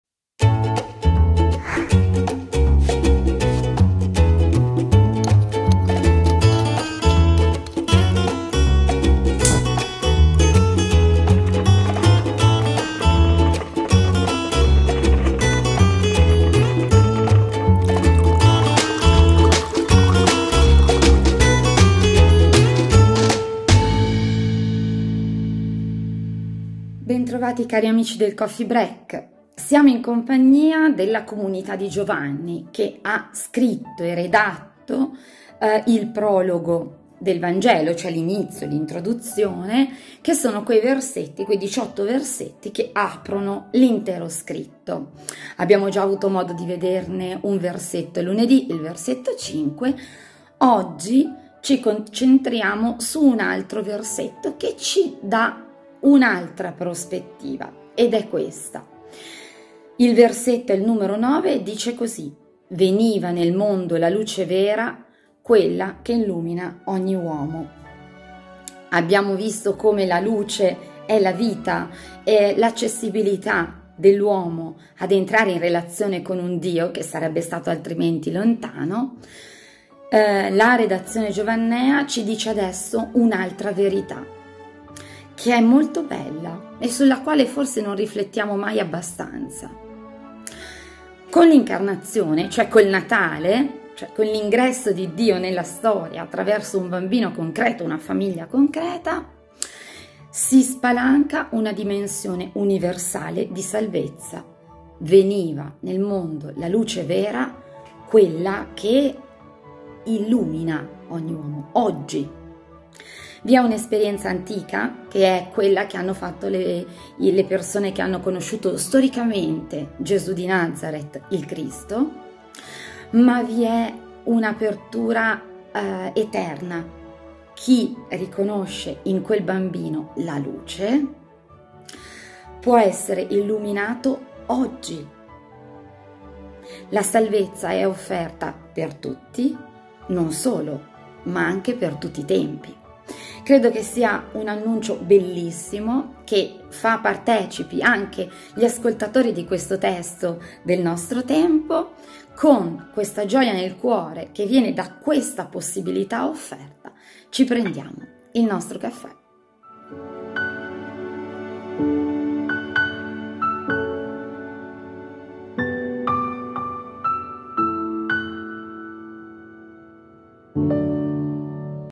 COFFEE BREAK (29 Dic. 2021)
COFFEE BREAK
una produzione Phrenhouse

CHIARA MARIOTTI
presenta ogni giorno un pensiero
degli uomini e delle donne della Bibbia

Gv 1,9